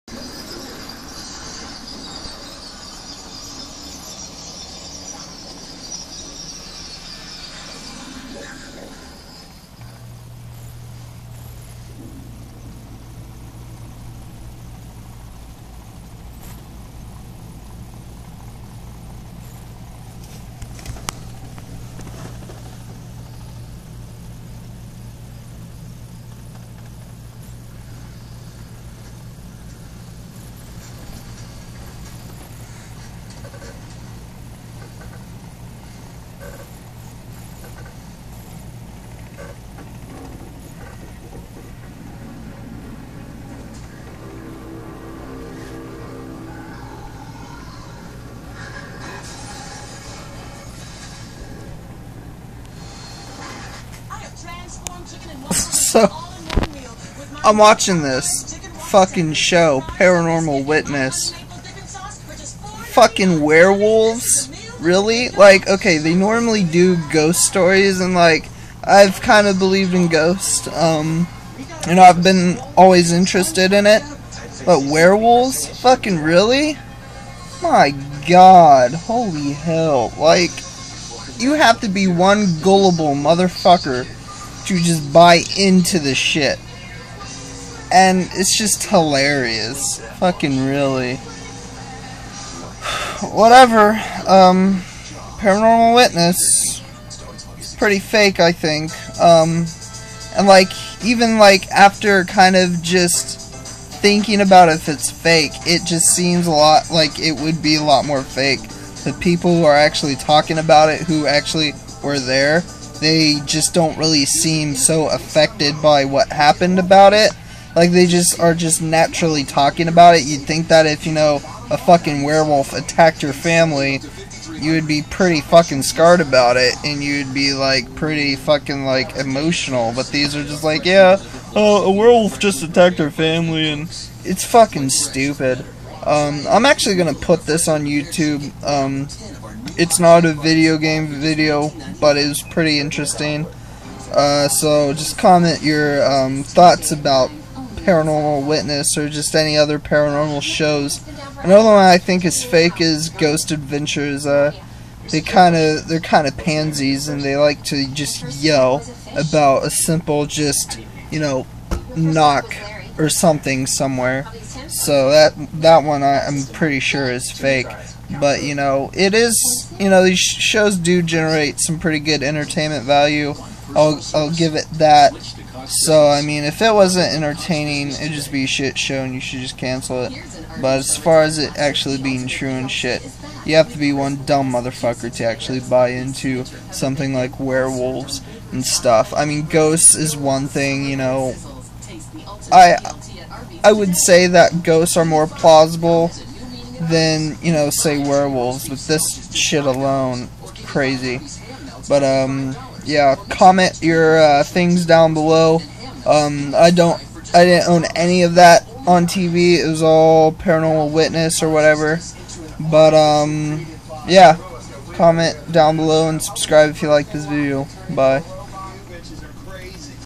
Paranormal Witness Fake?
When Paranormal Witness usually is on they share a pretty dark ghost tale that in itself is pretty extreme under "normal" paranornal circumstances, but Warewolves??? Comment what you think!